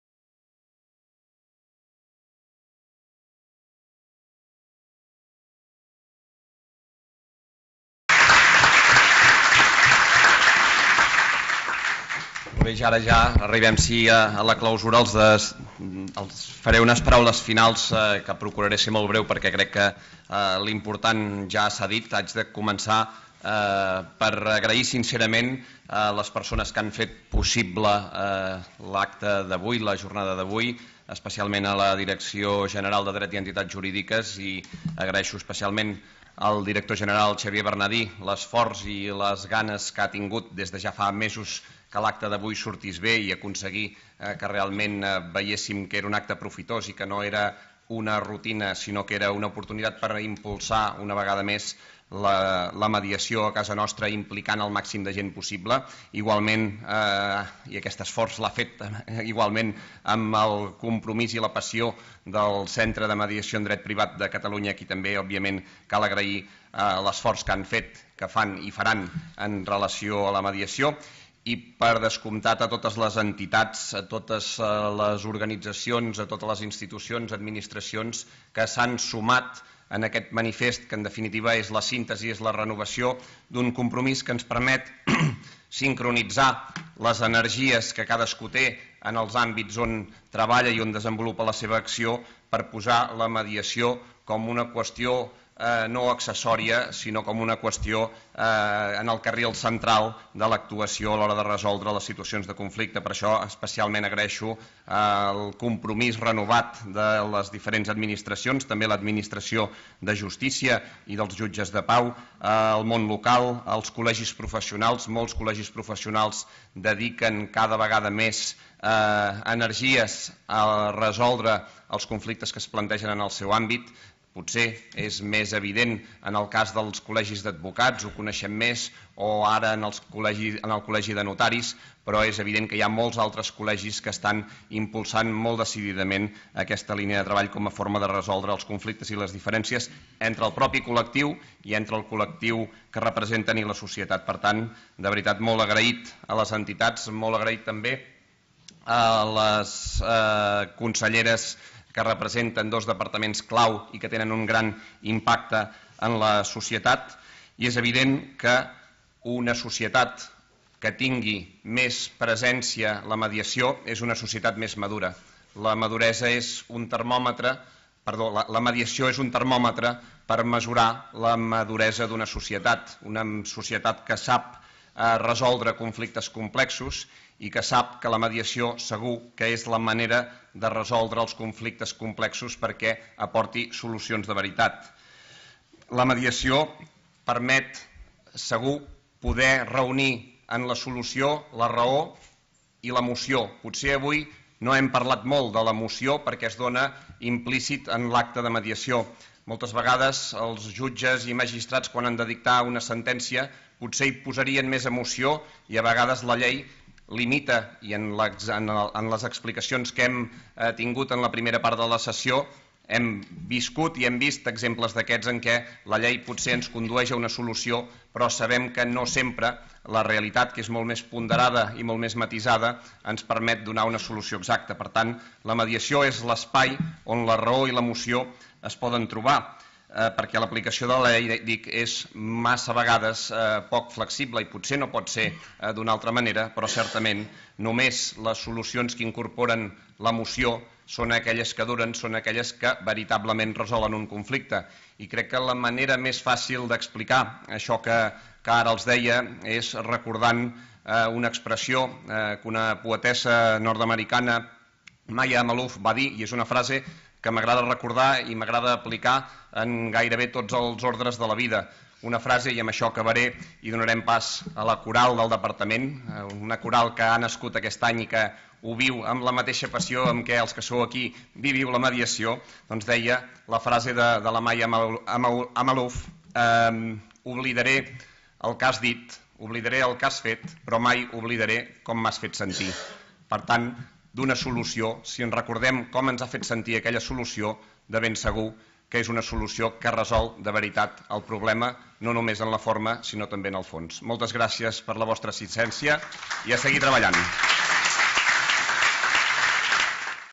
170121 Cloenda de l'acte commemoratiu del Dia Europeu de la Mediació
Acte commemoratiu del Dia Europeu de la Mediació
Cloenda a càrrec de Carles Mundó i Blanch, conseller de Justícia
Centre d'Estudis Jurídics i Formació Especialitzada, 21 de gener de 2017